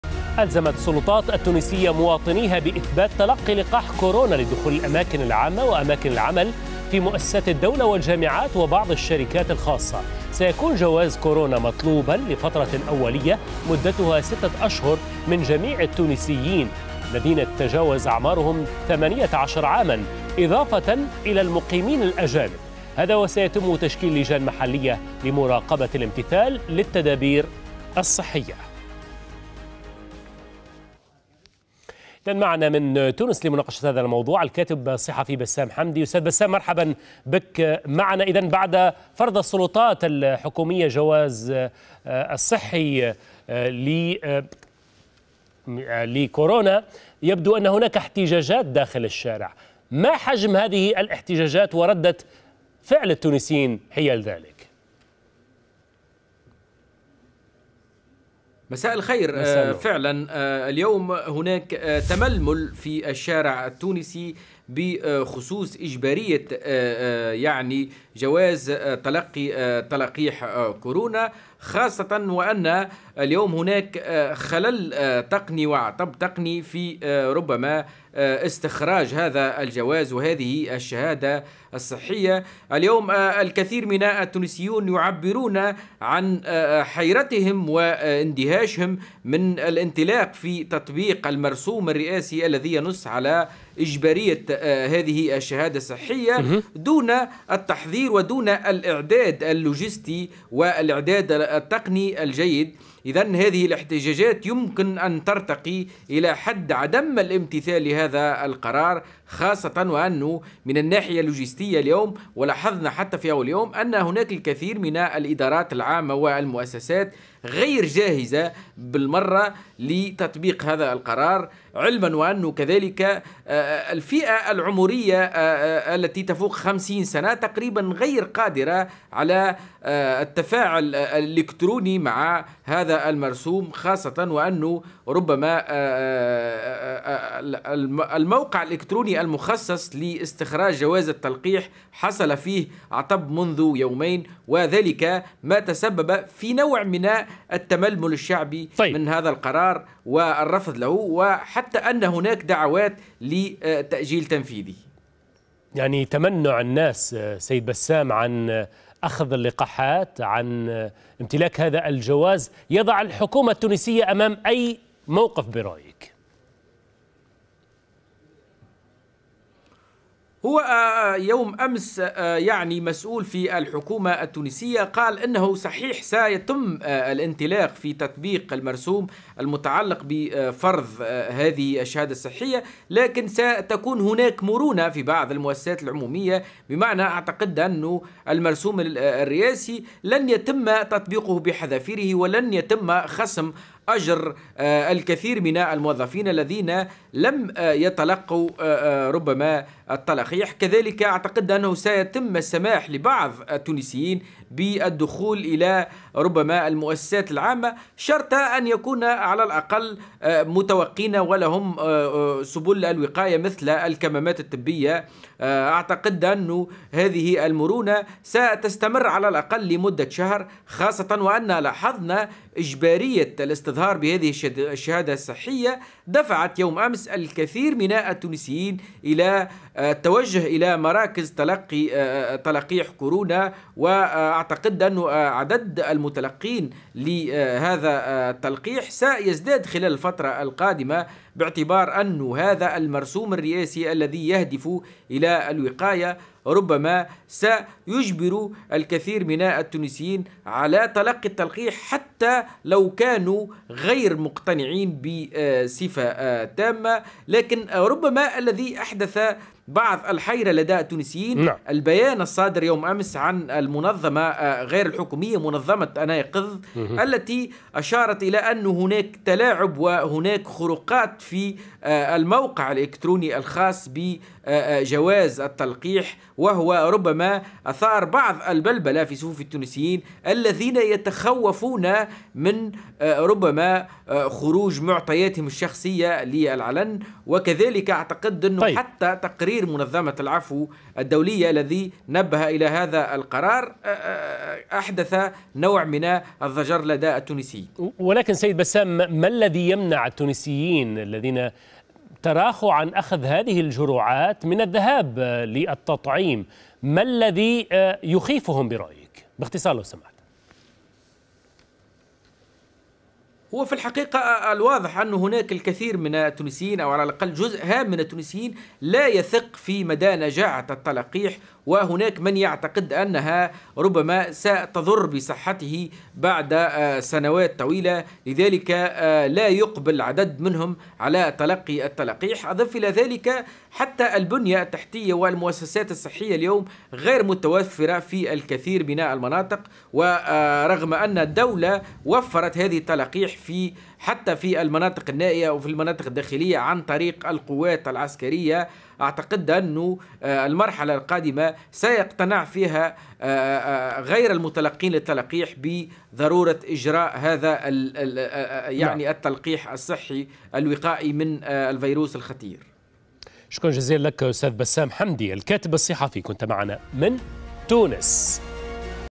تونس تفرض جوازات التطعيم لدخول الأماكن العامة
ألزمت السلطات التونسية مواطنيها بإثبات تلقي لقاح كورونا لدخول الأماكن العامة، وأماكن العمل في مؤسسات الدولة والجامعات وبعض الشركات الخاصة.
وسيكون جواز كورونا مطلوبا لفترة أولية مدتها ستة أشهر من جميع التونسيين الذين تتجاوز أعمارهم ثمانية عشر عاما ، إضافة إلى المقيمين الأجانب.
هذا وسيتم تشكيل لجان محلية لمراقبة الامتثال للتدابير الصحية.